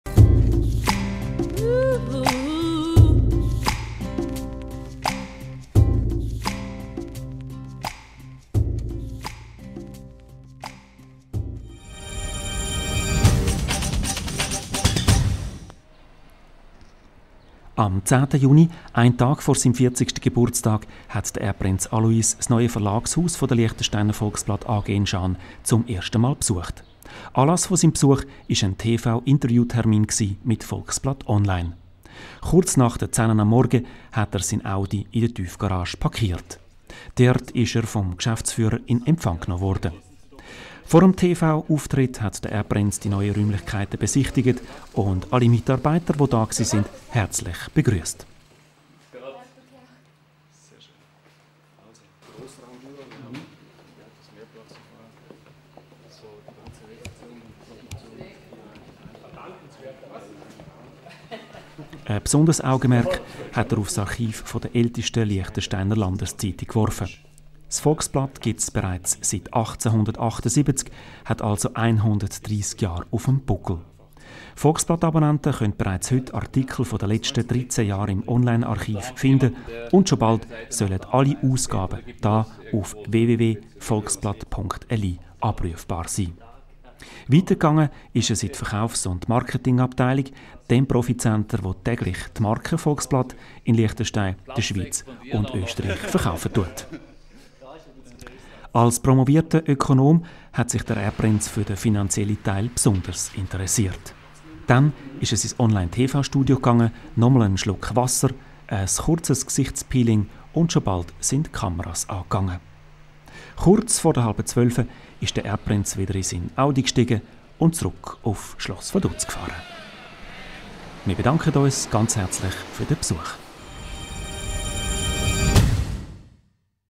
Erbprinz Alois von und zu Liechtenstein auf Besuch
Der Erbprinz besuchte einen Tag vor seinem 40. Geburtstag die modernste Tageszeitung Liechtensteins, das "Liechtensteiner Volksblatt".